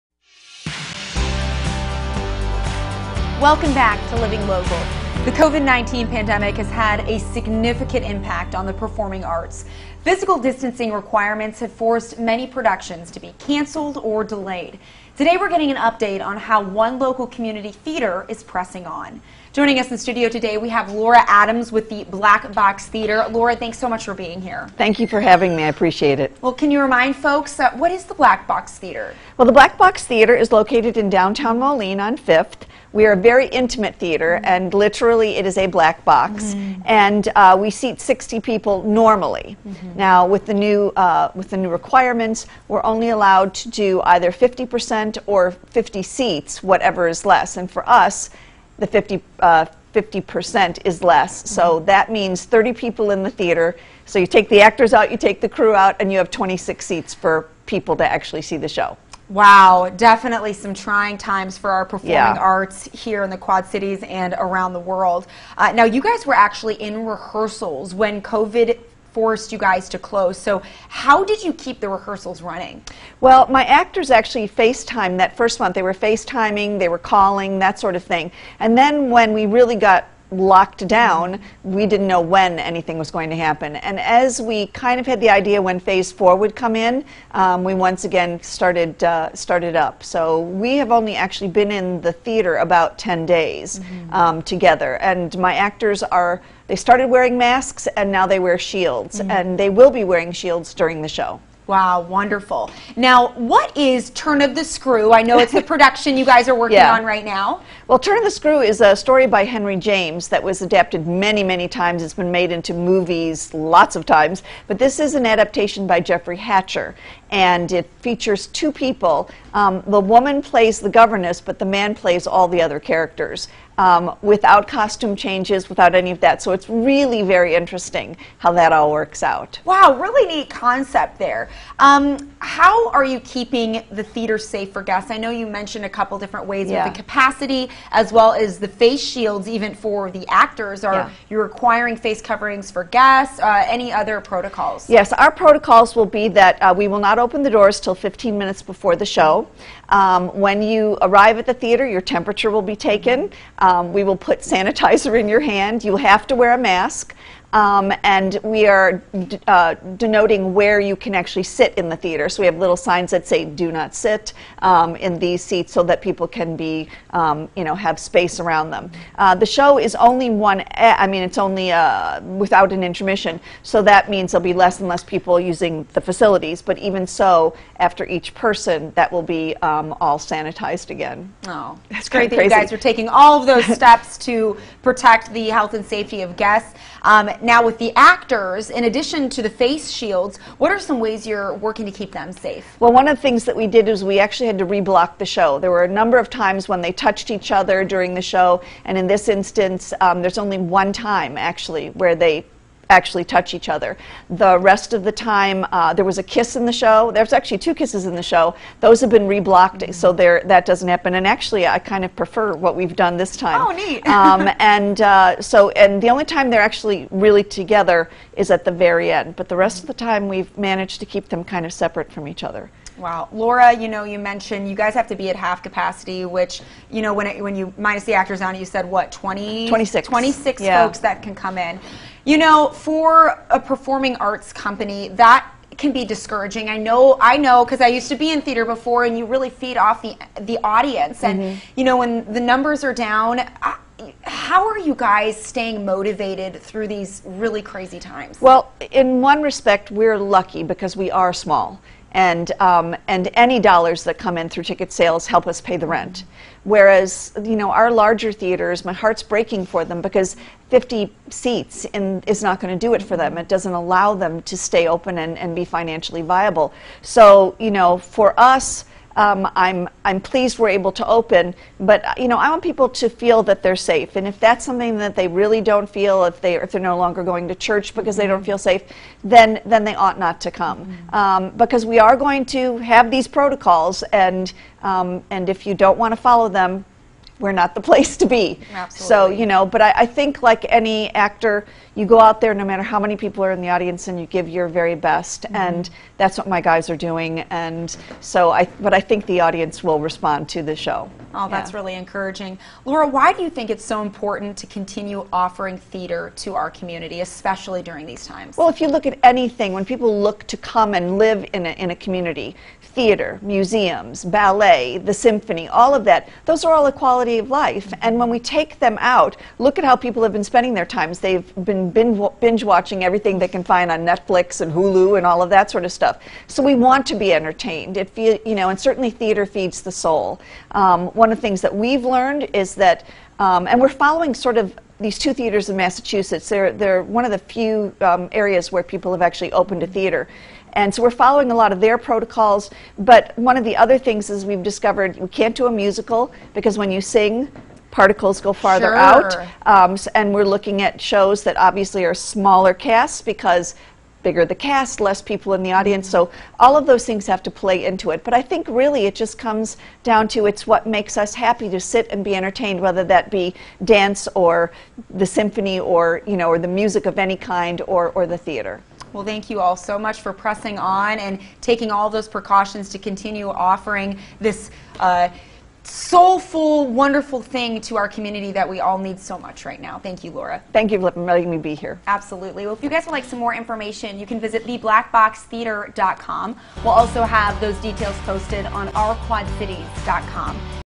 Black Box Theatre | “Turn of the Screw” Show Opening
The COVID-19 pandemic has had a significant impact on the performing arts. Physical distancing requirements have forced many productions to be cancelled or delayed. Today we’re getting an update on how one local community theatre is pressing on.